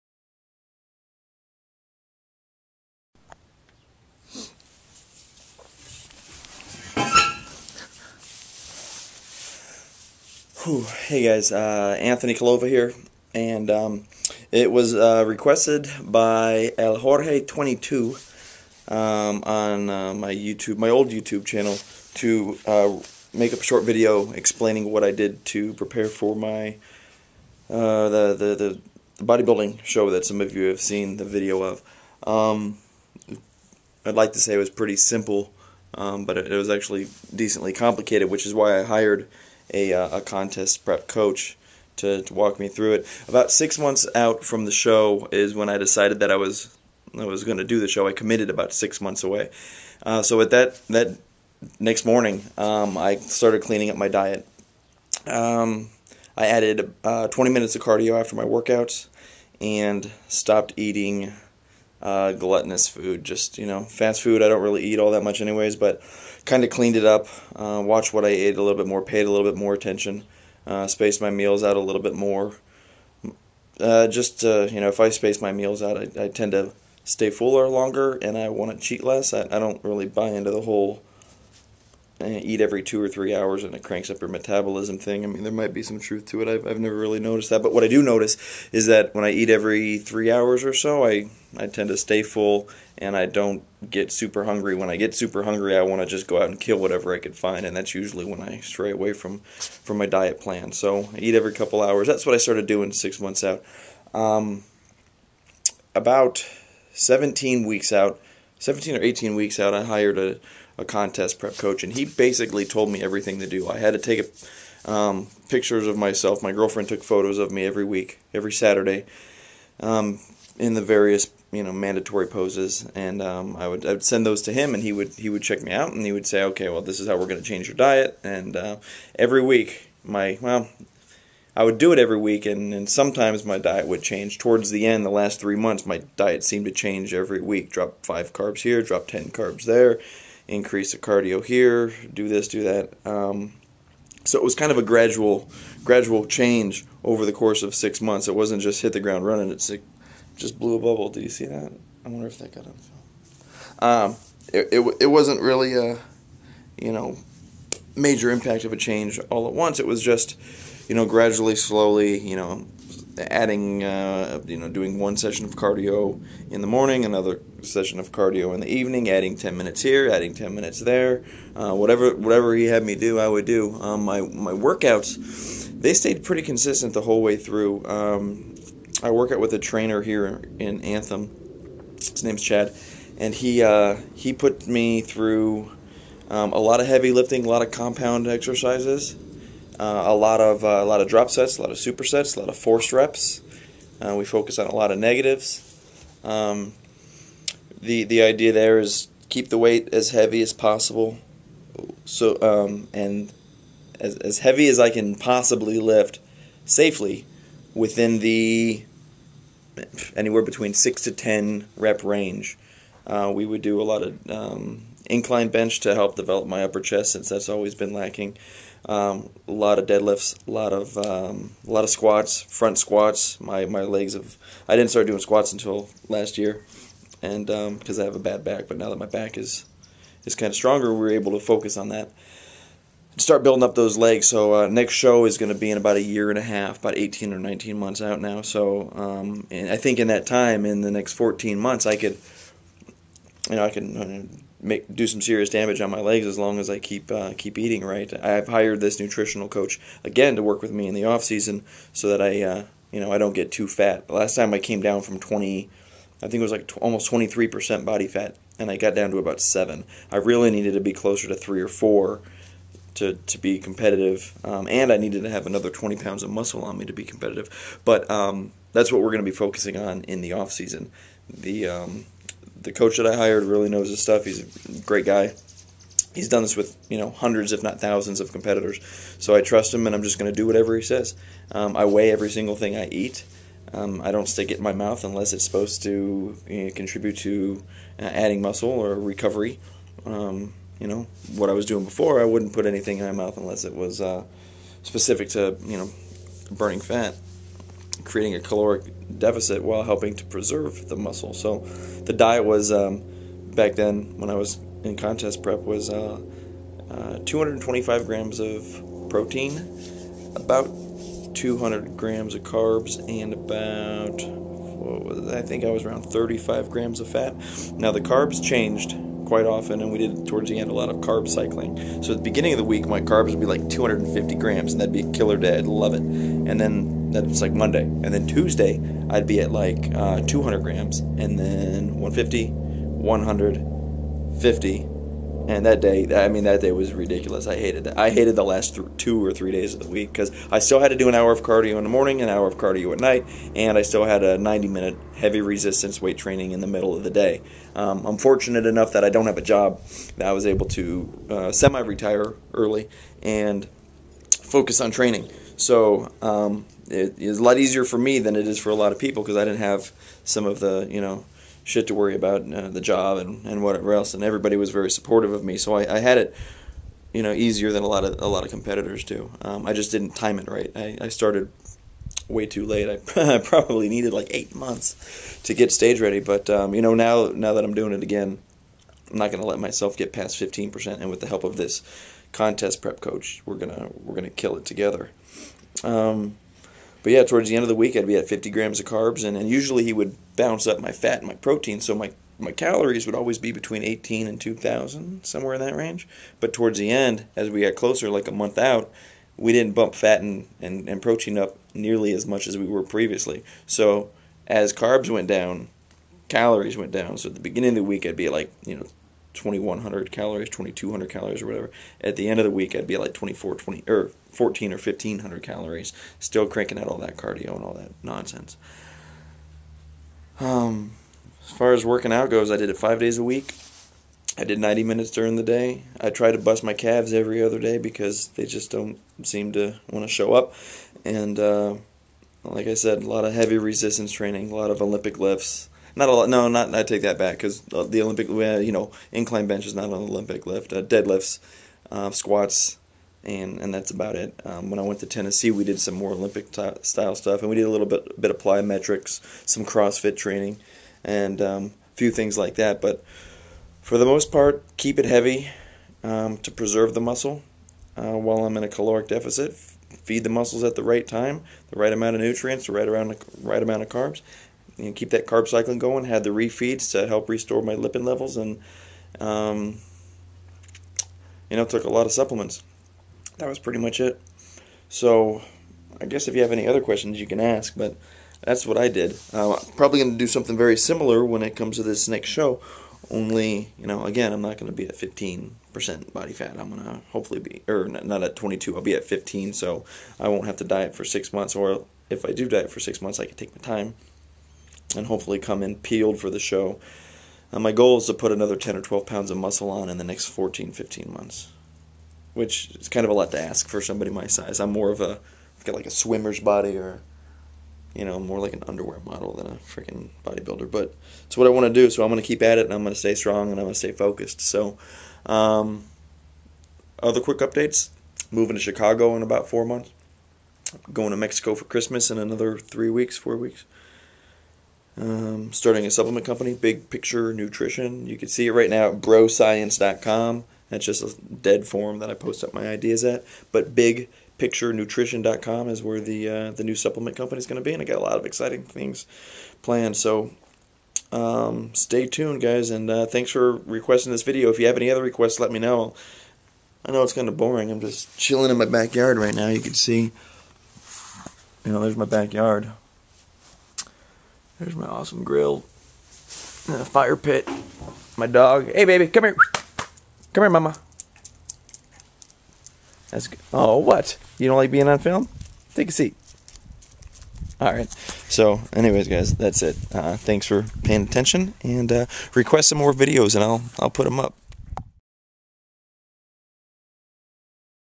Anthony Collova Answers Contest Prep Questions From eljorge2
Anthony Collova going over some simple pre contest questions